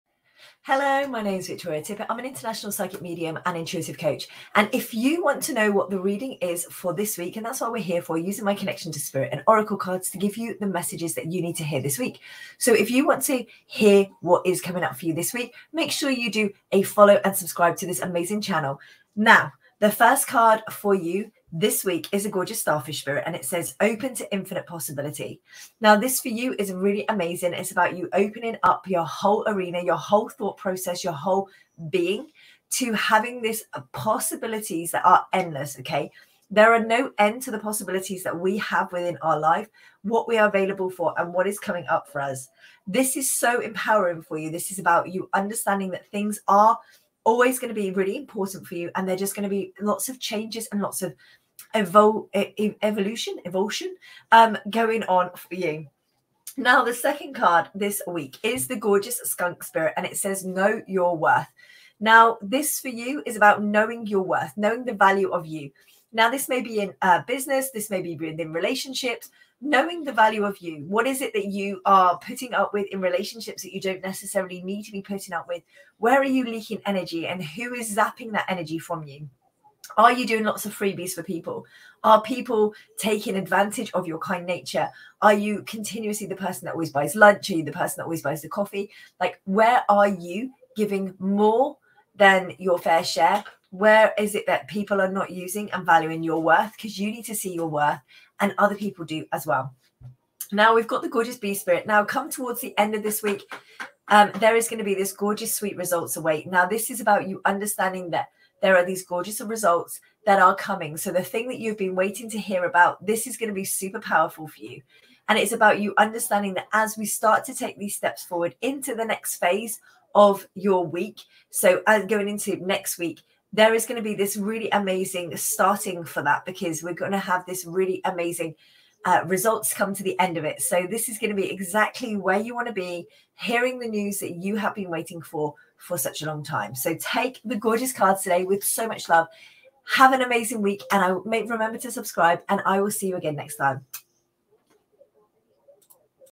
Awaken your inner warrior 🌟‌
I’m so excited to use my connection to spirit and animal oracle cards to give you the messages you need to hear this week! 🔮

Keep reading to find out how you can have an amazing week ahead. ⤵️

Spirit wants you to be fearless and bolder than you’ve ever been before. ⚡️ The only way to reach new levels is to get out of your comfort zone and take risks… and right now is the time to do so! The universe will reward you for stepping into the warrior version of yourself instead of the worrier.

Remember that you are a spiritual being having a human experience. Sometimes the human stuff is going to knock you down and that’s OKAY. It’s important that we have this duality of light and dark, good and bad, easy and difficult, so that we learn how to appreciate life and find the power of gratitude each and every day. 🙏🏼🙏🏼

Take 2 minutes to listen to your full message from spirit here